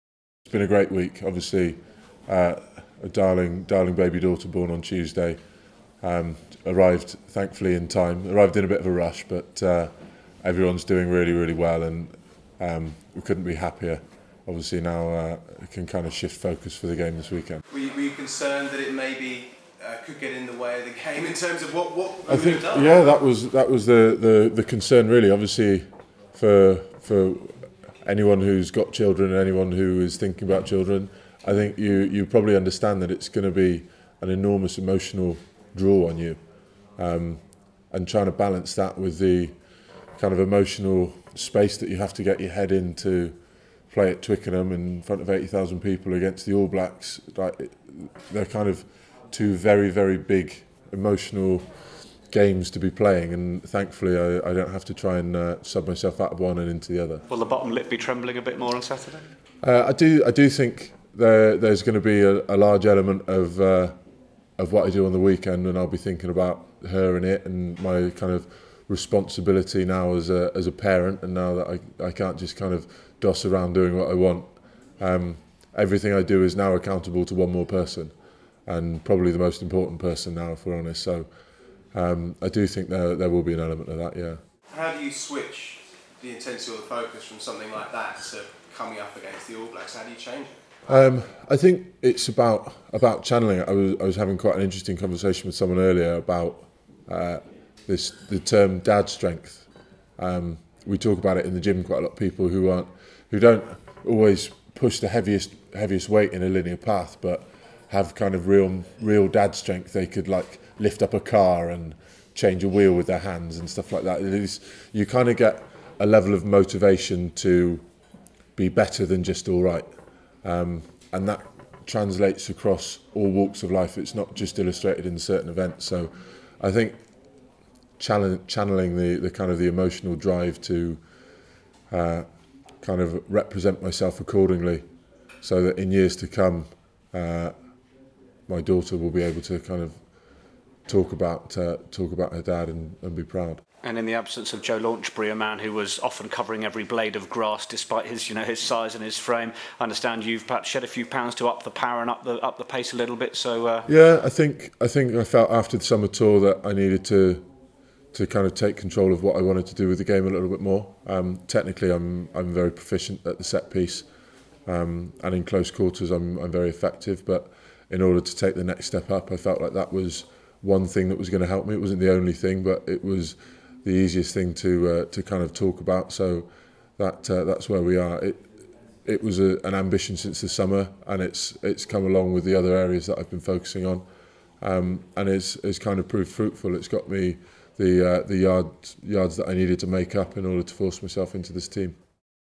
Dave Attwood faces New Zealand after birth of baby daughter
Bath lock Dave Attwood will face the All Blacks on Saturday at Twickenham having become a father this week to a baby daughter. He spoke to media about his feelings ahead of the match.